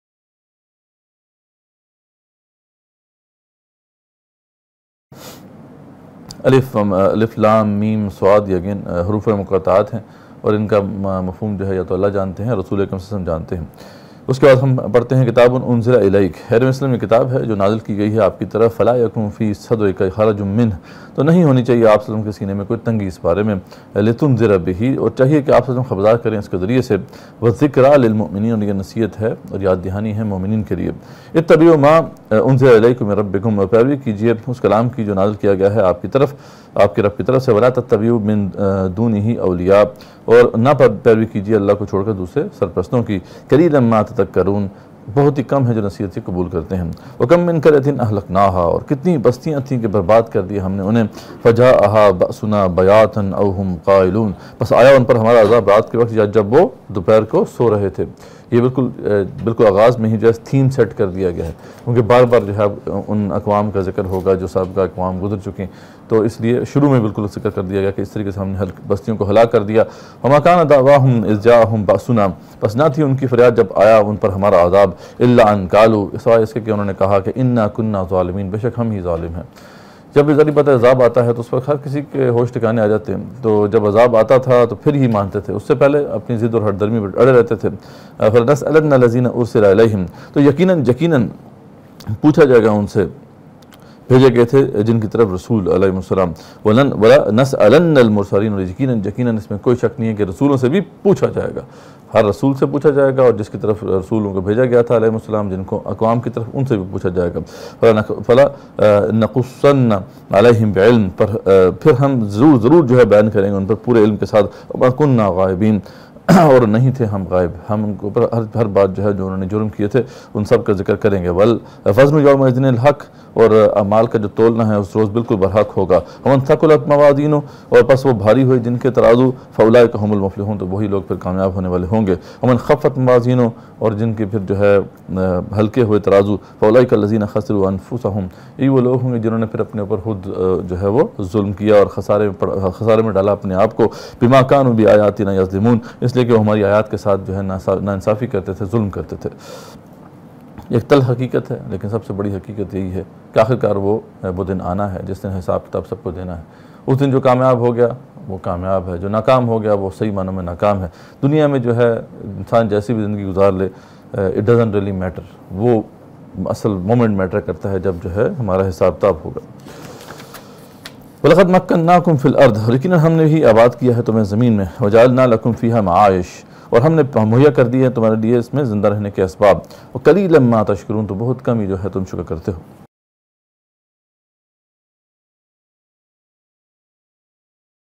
13. Surah Al A'raf Ayat 1 to 10 (Para8 Part5 Week3)
دورہِ قرآن کورس (سمر) 2021 از جنب مدثر راشد صحاب - نووا ایجوکیشن (روشنی کے راستہ)

Dora-e-Quran Course (Summer) 2021 by Janab Mudassir Rashid Sahab - Nwaa Education (Road to Enlightment)